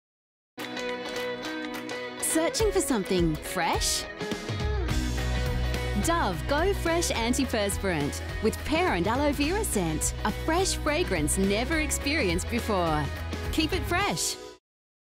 New Dove Go Fresh Pear & Aloe Vera Deodorant
Searching for fresh? Discover the unexpected freshness of Dove Go Fresh Pear & Aloe Vera anti-perspirant deodorant.